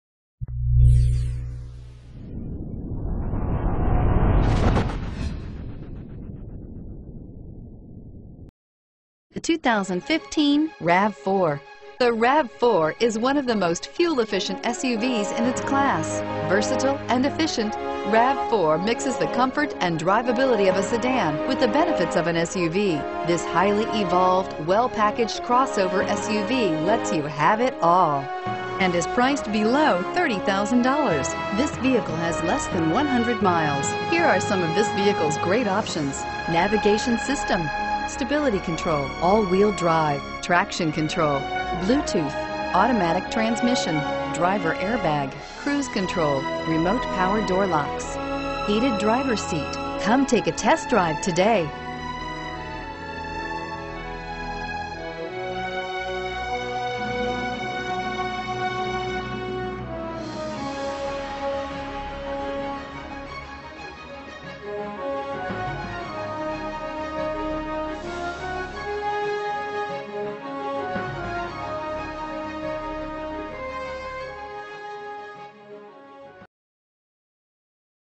2015 Toyota RAV4 Kalamazoo MI A7418
2015 Toyota RAV4 Limited

Metro Toyota
5850 Stadium Drive

Pick up this Pyrite_mica 2015 Toyota RAV4, available today at Metro Toyota. Featuring Automatic transmission, it has 1 miles. This could be the one you've been searching for.